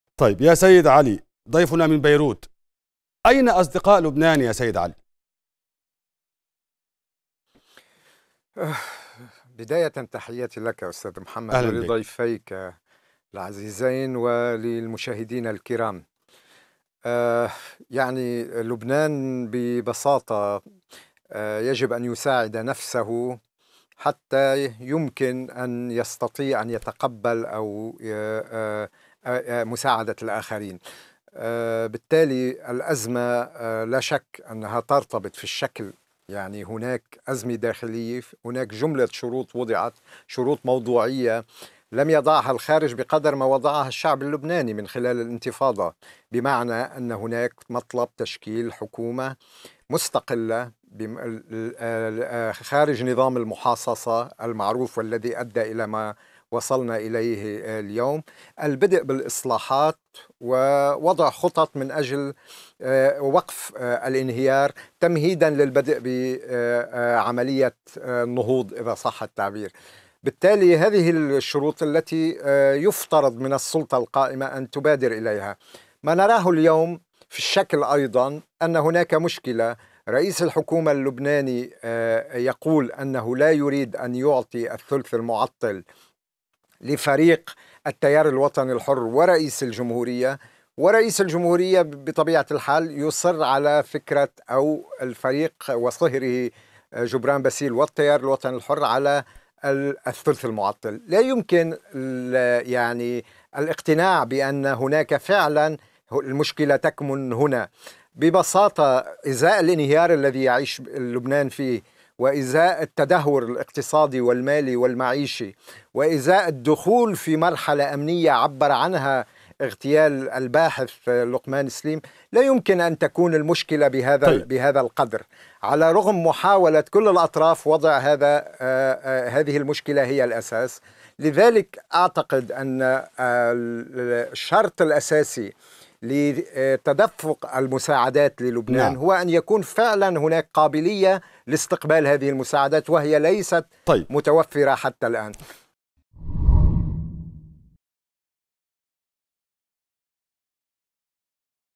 صحفي: هناك مطلب جماهيري بتشكيل حكومة لبنانية مستقلة بعيدا عن نظام المحاصصات
منذ بداية الأزمة في لبنان ، طرحت فرنسا  مبادرة تضمنت تشكيل حكومة كفاءات غير حزبية ، تقوم بإجراء الإصلاحات المطلوبة ، و حينها ستقوم فرنسا بحشد الدعم الاقتصادي لإنقاذ لبنان من عثرته .
لكن وبمرور الوقت لم يجد جديد و ظل كل طرف متمسكا بموقفه ما جعل كثيرين يؤكدون أن المبادرة الفرنسية تترنح ، و ربما ذلك هو ما دفع باريس إلى التأكيد على أنه لا تراجع عن مبادرتها ولا تغيير في بنودها .
فهل سيستمر الدعم  الخارجي للبنان ، أم سيتلاشى صداه  نتيجة التعنت وتبادل الاتهامات ؟

للاجابة على تلك التساؤلات مع ضيوف قناة الغد، من  بيروت علي الأمين رئيس تحرير موقع جنوبية و من باريس  أستاذ التواصل الاستراتيجي والعلاقات الحكومية الدكتور نضال شقير  ، و من دبي المحاضر بالجامعة الأمريكية في بيروت الدكتور مكرم رباح